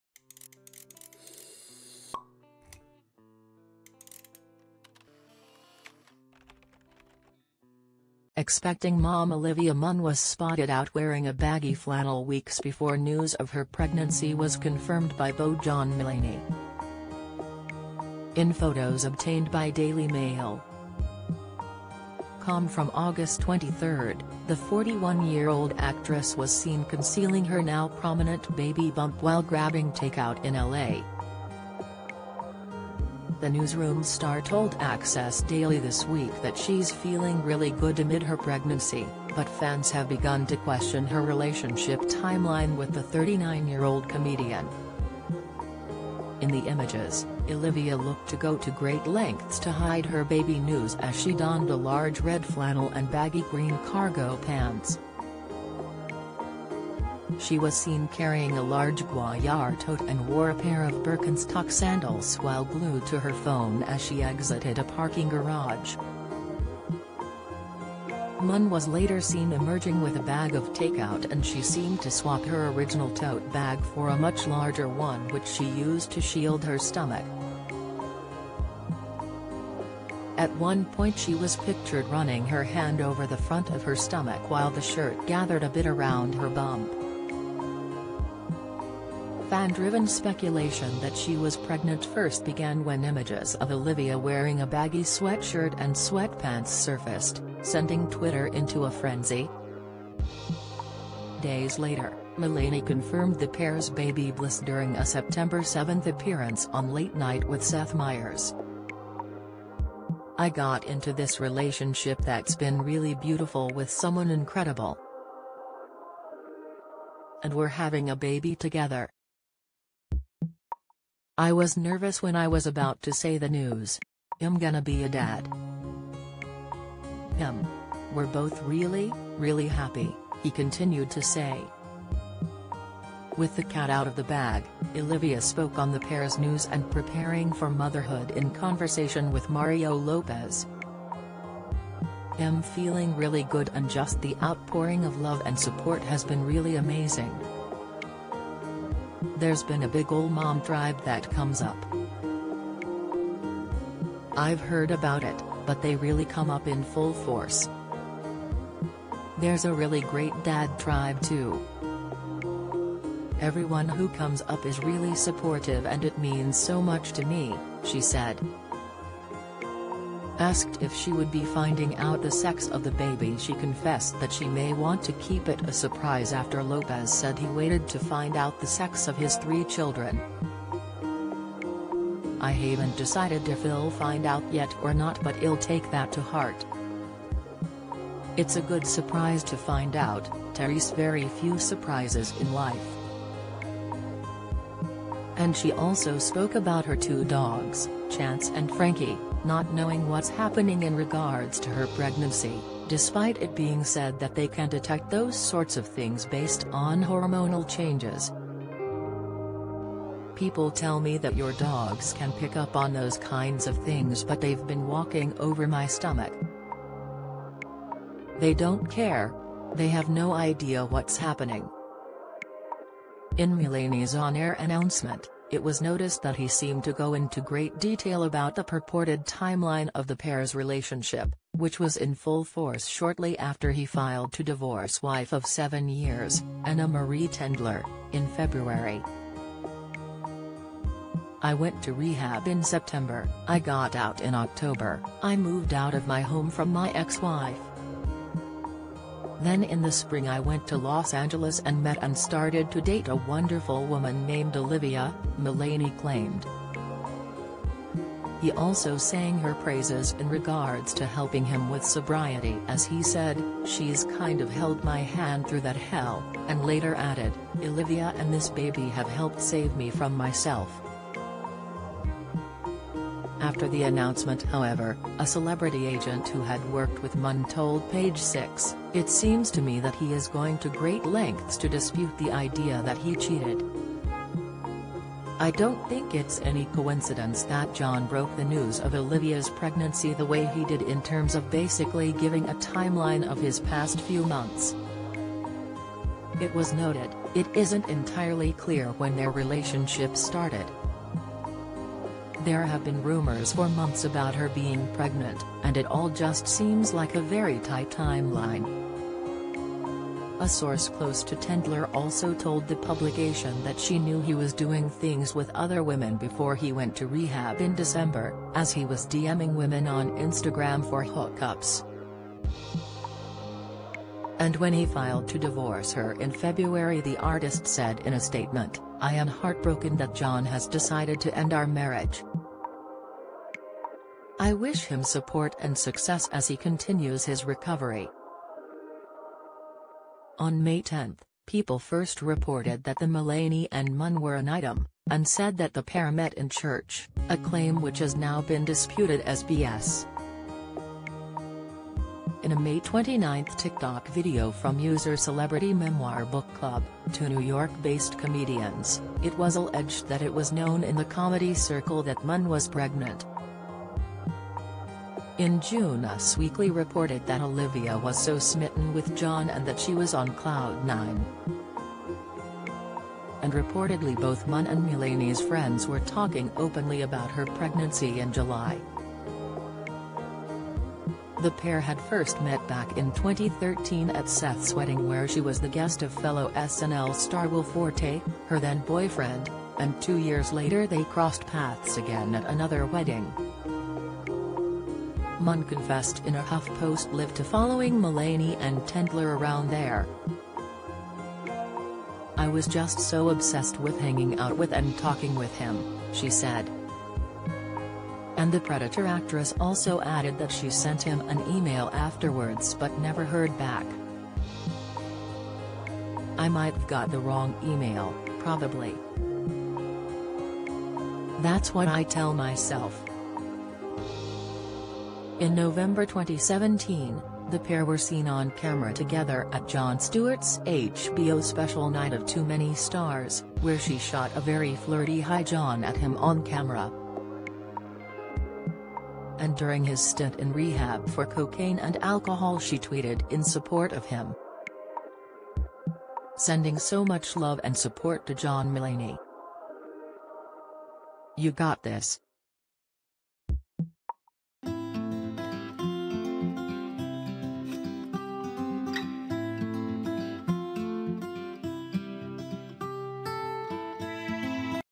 Pregnant Olivia Munn covers her bump in a flannel amid fan speculation about John Mulaney timeline 9
Pregnant Olivia Munn covers her bump in a flannel amid fan speculation about John Mulaney timeline 978769